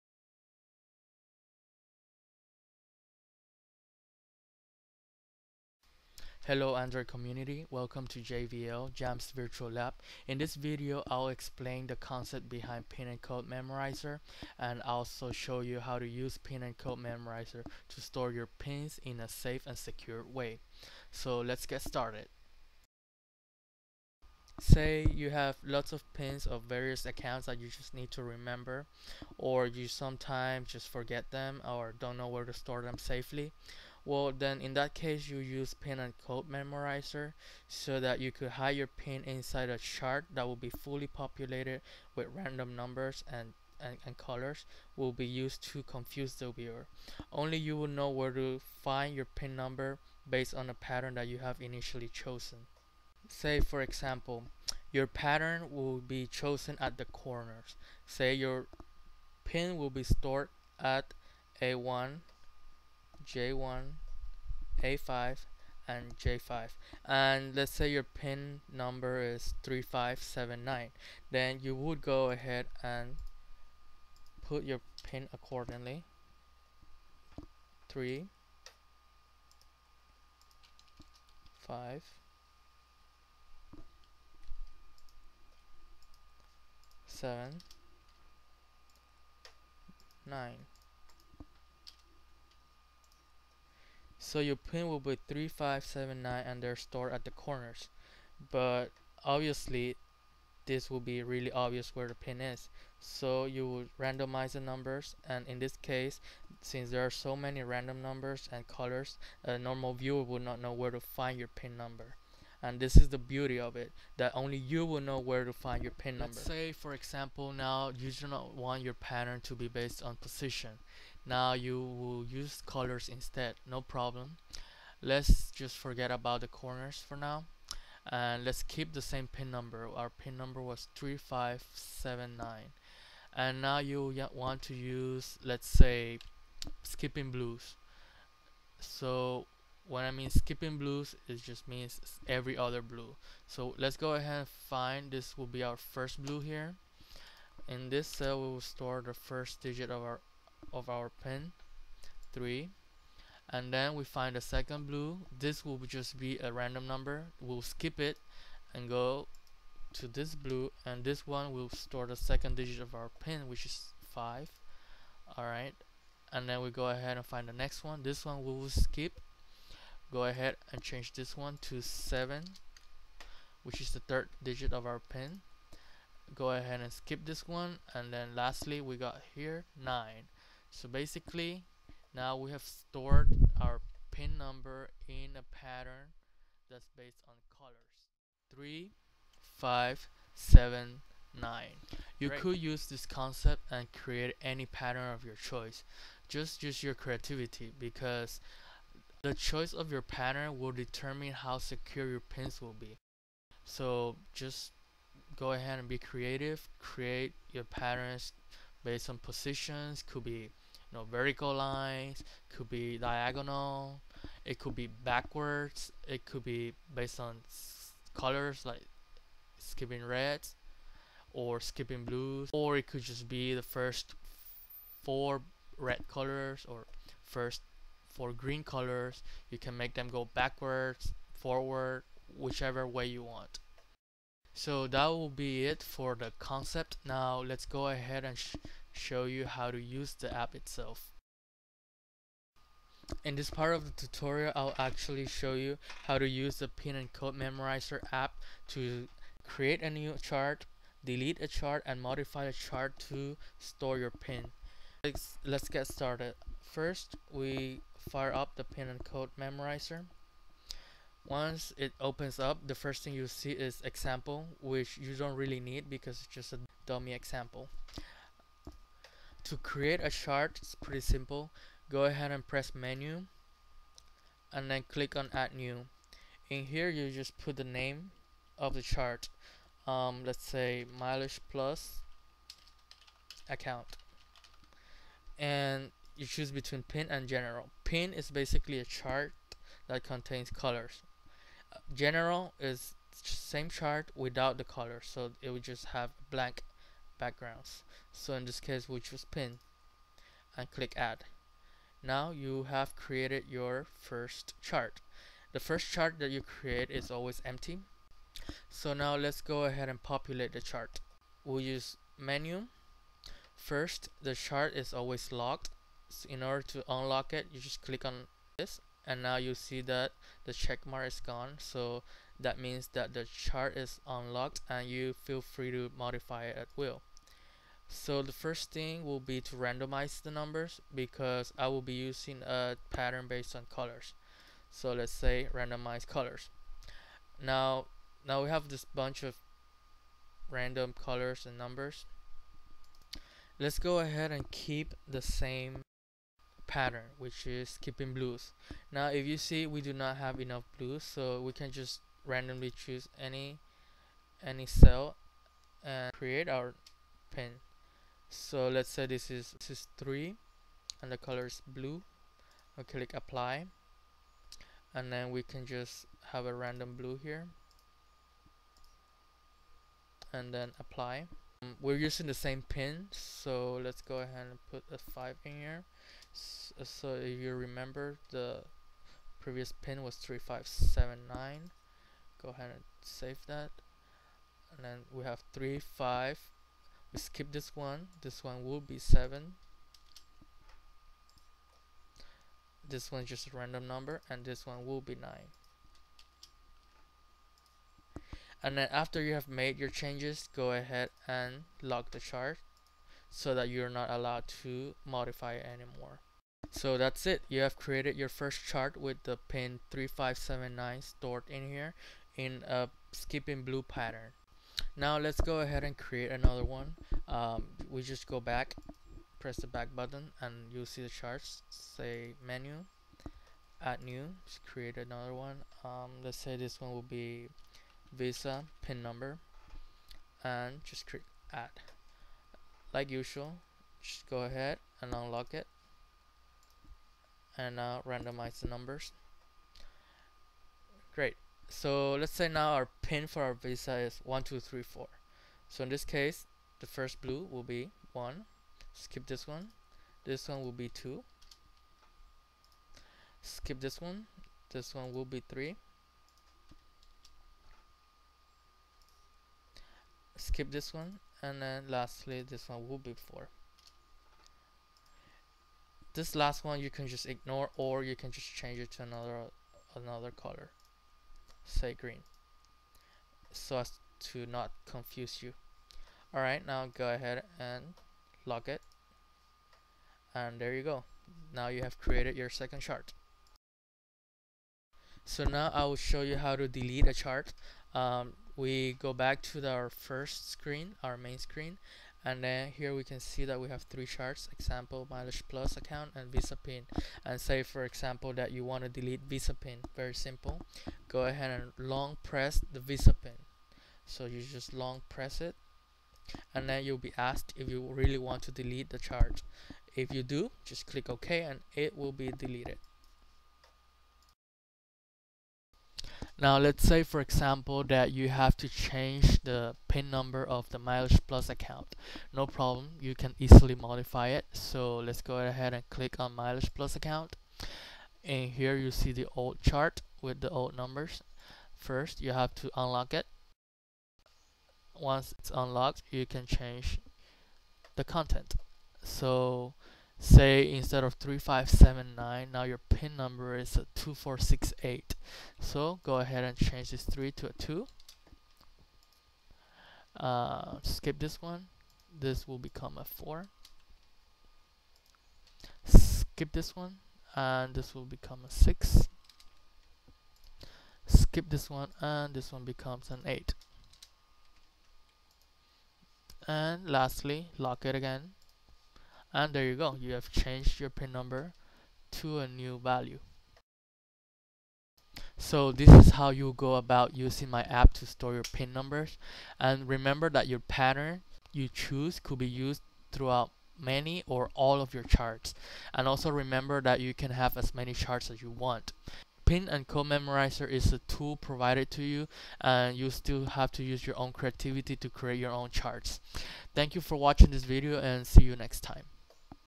Pin & Code Memorizer: HowTo Video
This video explains the concept behind Pin & Code Memorizer and shows you how to use it to store Pin's and Codes.